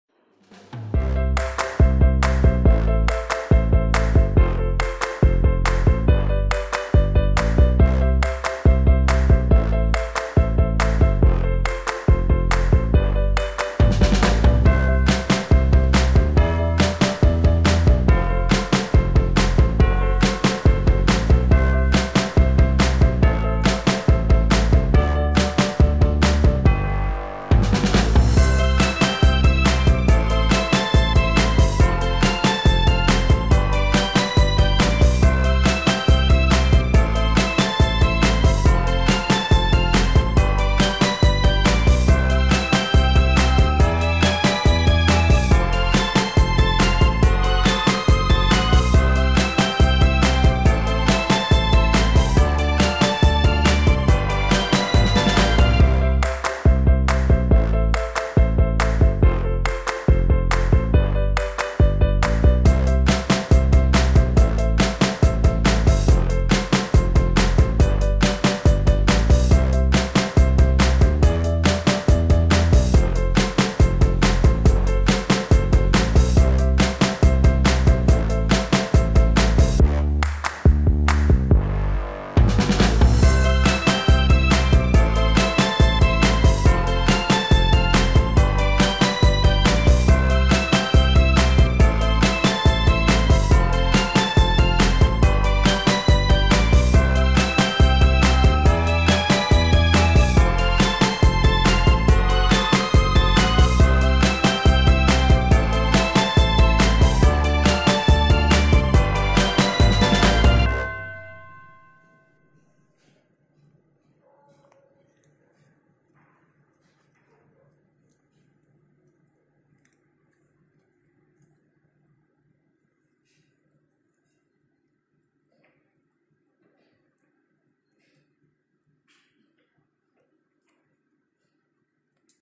İmece kuran kediler öğle yemeğinde
Topluca öğle yemeği yiyen kedi grubu